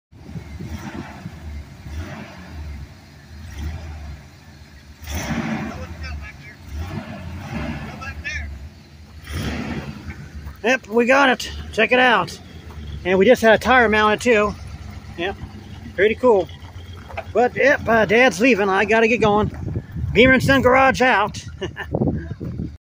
2001 Toyota Corolla CE hit hard in the front Hooked up and heading back home yeah winning again
2001 Toyota Corolla CE hit hard in the front Hooked up and heading back home yeah winning again